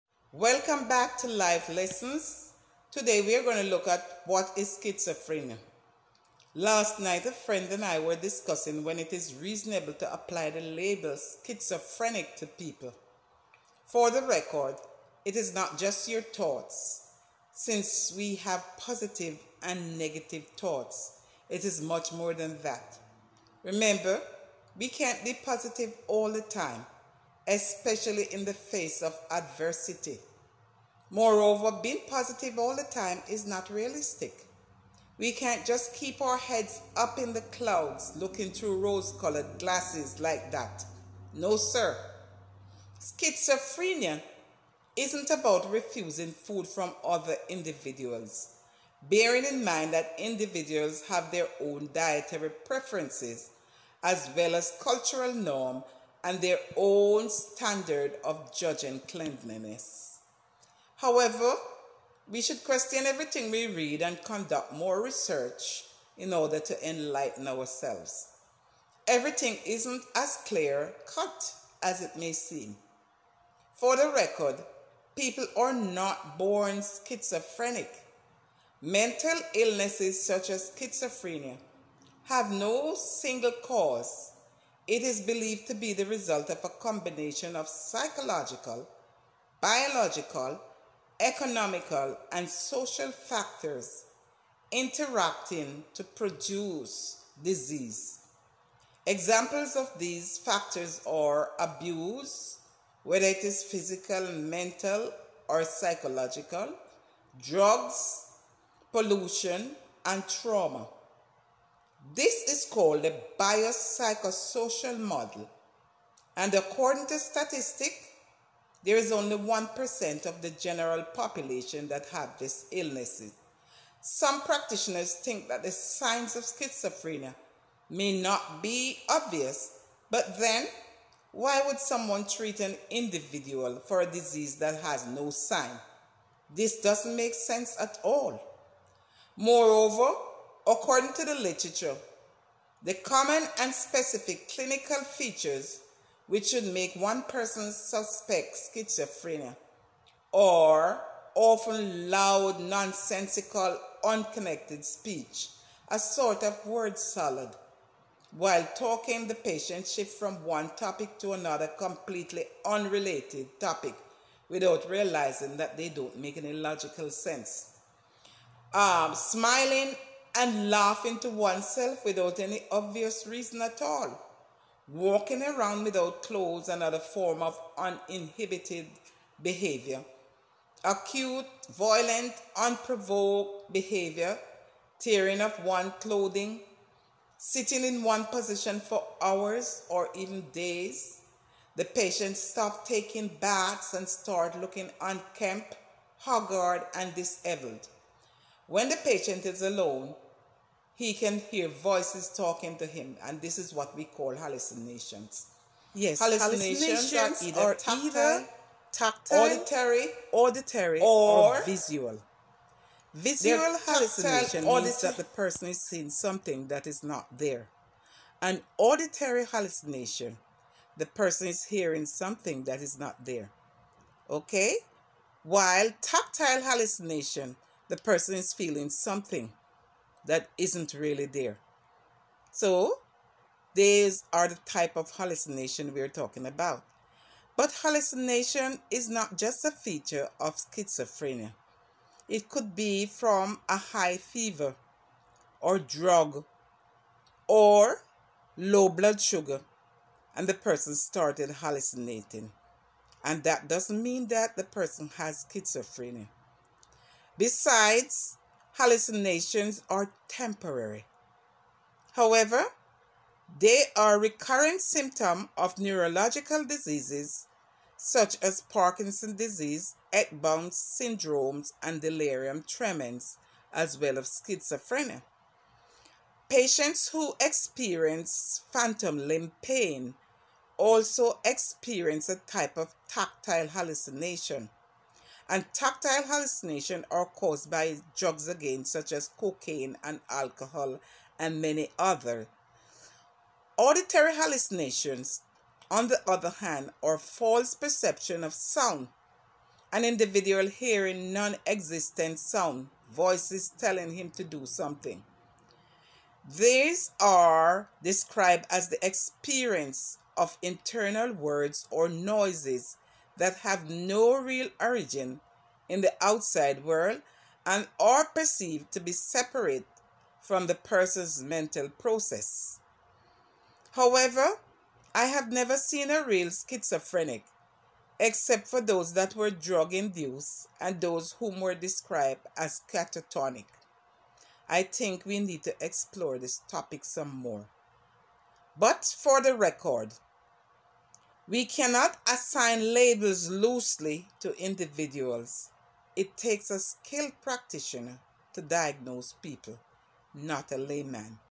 What is Schizophrenia? The distorted picture that you are viewing is an art effect, not an illness.
The distorted picture that you are looking at, is an art effect, not an illness. Manipulating an individual's environment by tactics such as gaslighting, doesn't play a part in diagnosing Schizophrenia.